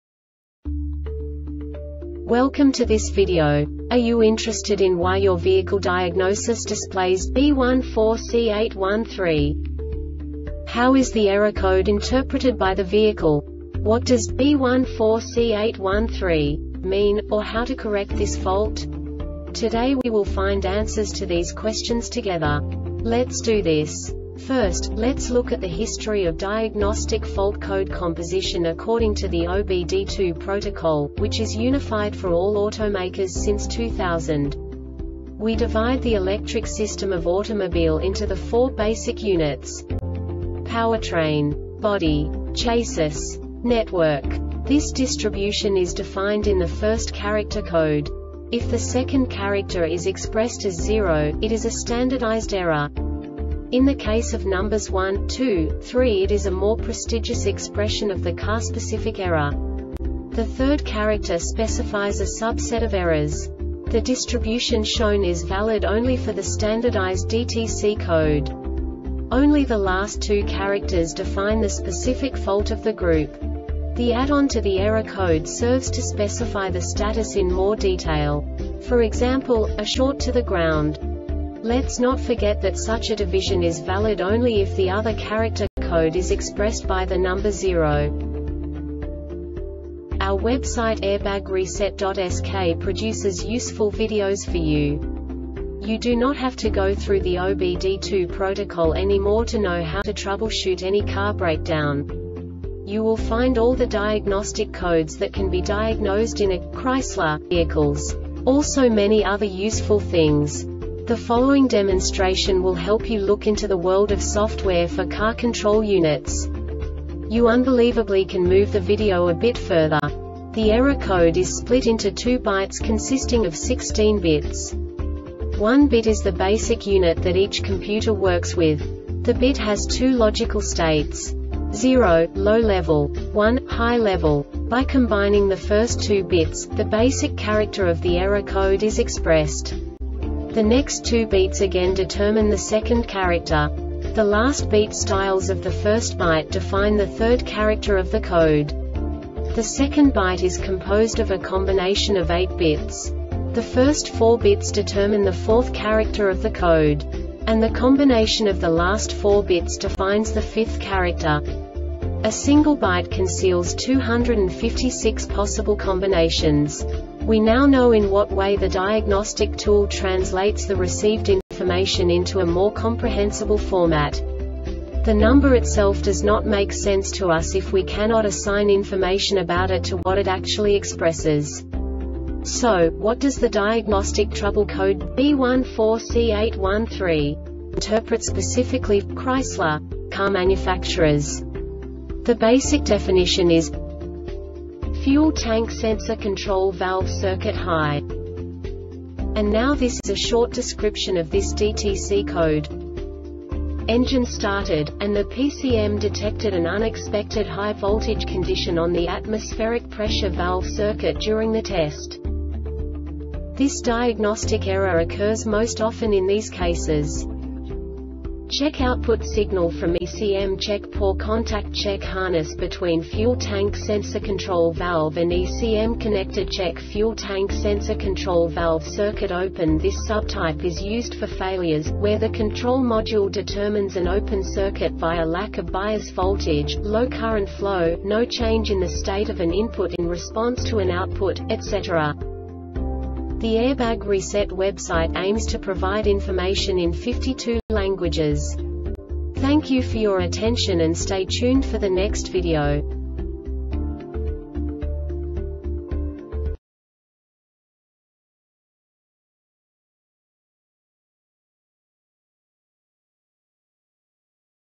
DTC Chrysler B14C8-13 Short Explanation
Contents:
0:21 Basic DTC analysis according to OBD2 protocol standard.
1:48 Insight into programming
2:58 A diagnostically understandable description of the Diagnostic Trouble Code
3:05 Basic error definition

Make:
Chrysler
Code:
B14C8 13
Definition:
Fuel Tank Sensor Control Valve Circuit High
Description:
Engine started,and the PCM detected an unexpected high voltage condition on the Atmospheric Pressure valve circuit during the test.
Cause:
Check output signal from ECMCheck poor contactCheck harness between fuel tank sensor control valve and ECM connectorCheck fuel tank sensor control valve
Failure Type:
Circuit Open